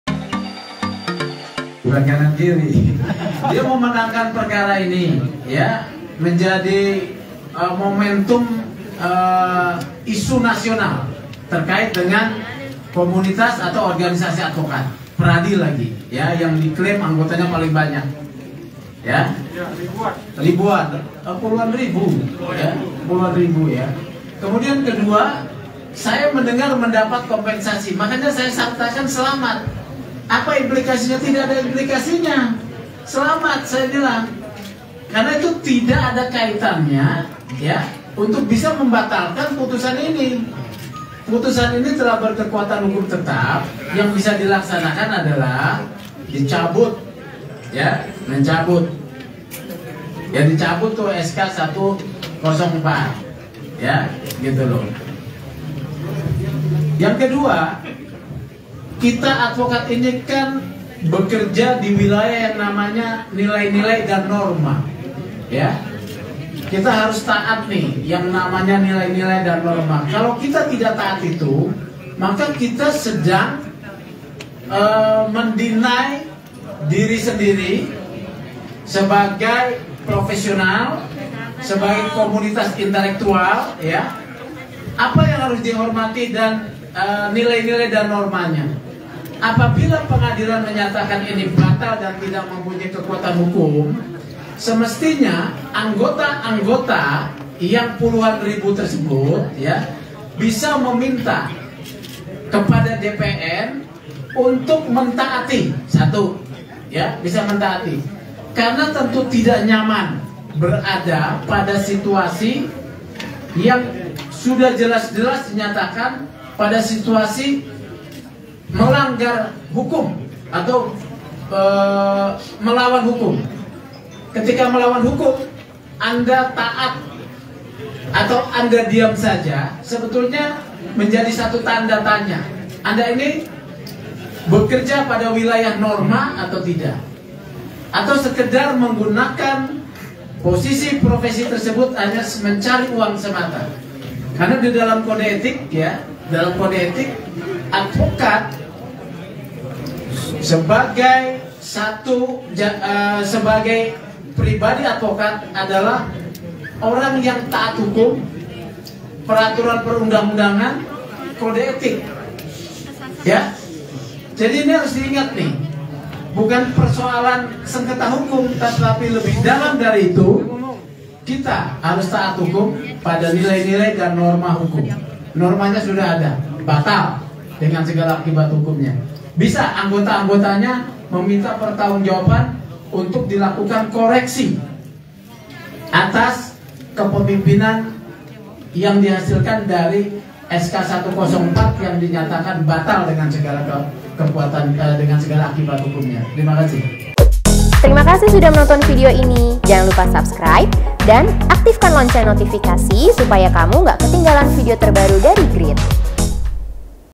Sekjen Dewan PERADI Membela Hotman Paris Yang Dituduh Oleh DPC PERADI Melanggar Kode Etik Advokat
Grid.ID

EDITORIAL OFFICE

ADVERTISING